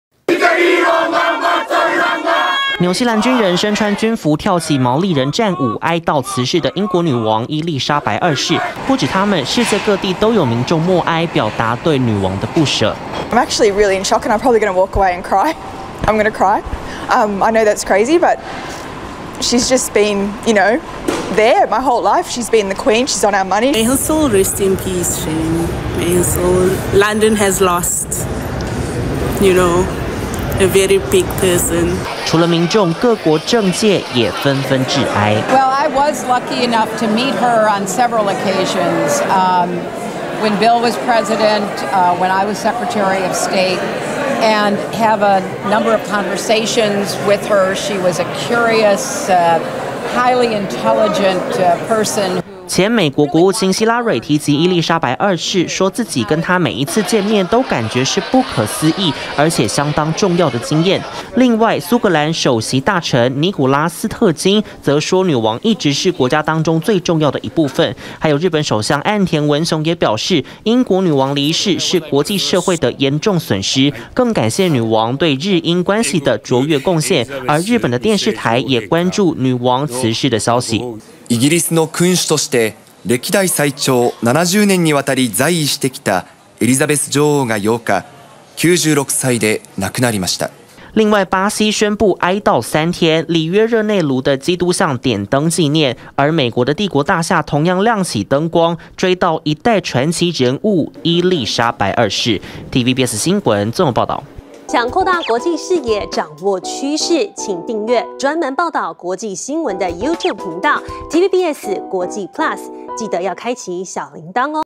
全球齊哀悼! 民眾掉淚不捨 各國政界憶女王｜TVBS新聞
英國女王伊莉莎白二世辭世，全球各地不少民眾哀悼，有人受訪時更是不捨的眼淚就在眼眶中打轉，另外各國政界也紛紛致哀，前美國國務卿 希拉蕊，以及日本首相 岸田文雄都表達不捨，而巴西則宣布哀悼三天 ，里約熱內盧的基督像點燈紀念，美國的帝國大廈也亮起紫色燈光，致敬這位傳奇人物。